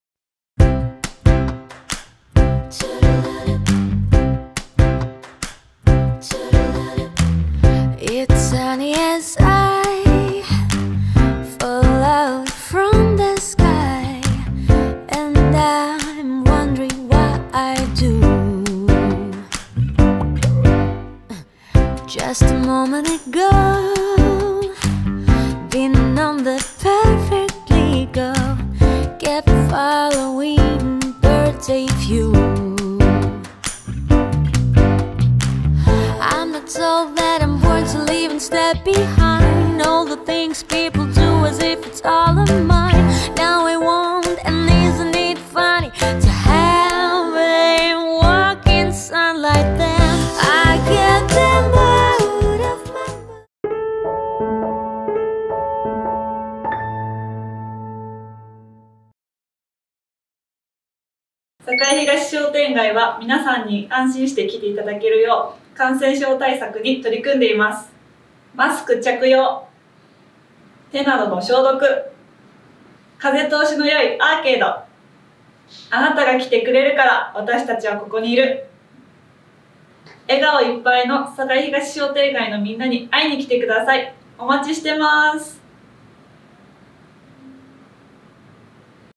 【公式】元禄寿司 堺東店 職人が握る旨いコスパ最高の元祖廻る回転寿司。【堺東商店街公式PRムービー】
■元禄寿司 堺東店

回転寿司の元祖！
元禄寿司が登場！
堺東店にも長年のキャリアをもつ職人さんが在籍。
旨い！これが回転寿司か！？と驚くほど旨い！
旬の食材も常に入れていて飽きさせない。
ガリもいい塩梅で美味しい！
茶碗蒸しにもさり気な隠しアイテムがイン！

動画に映っている以外にも隠れた人気の"カニサラダの軍艦巻き"もお勧めだ！

１人でも入りやすく、お持ち帰りもある。
堺東に来たなら是非一度寄ってみてください。

堺東商店街の情報を素早くキャッチ！

※この動画は堺東商店街がコロナ感染症対策をしているPRをする目的で作成されています。

是非、皆様ご来店のほどお待ちしております。

制作: 堺東中瓦町商店街振興組合

プロデューサー、撮影、映像編集:澤田 嘉寛（古墳レコードズ）